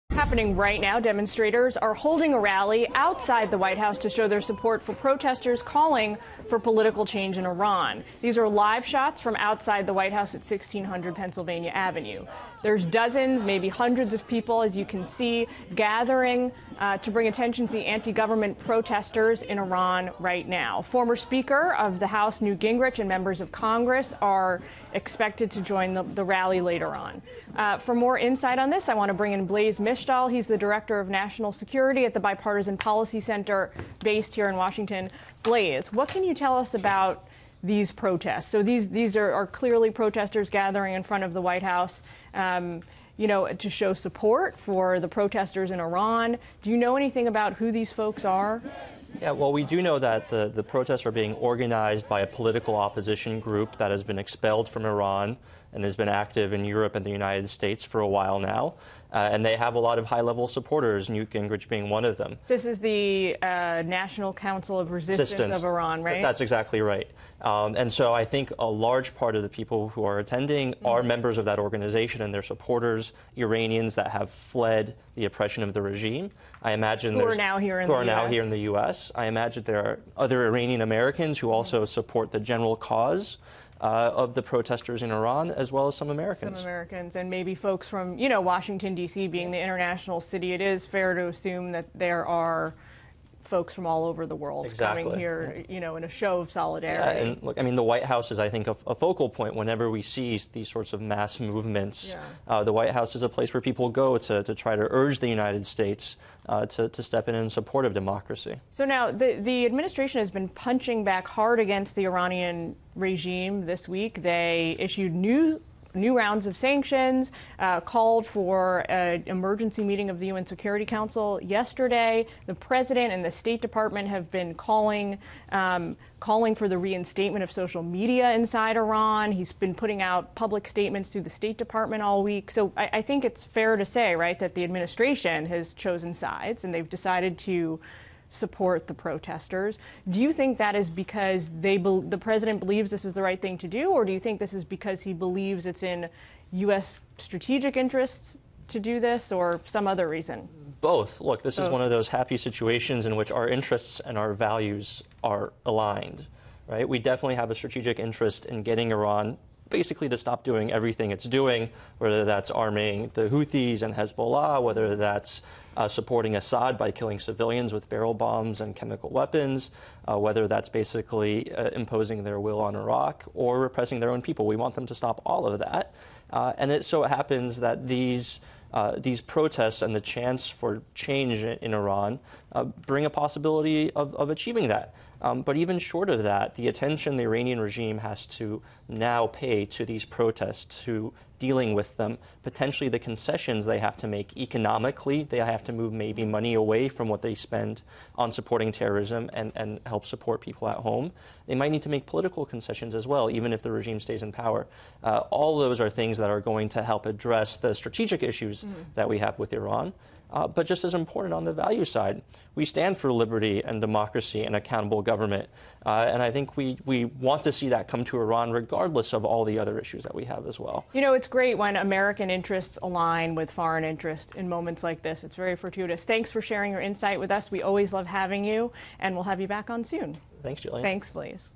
Rally held outside White House in support of Iran protests
Blaise Misztal, director of the national security program at the Bipartisan Policy Center, shares insight.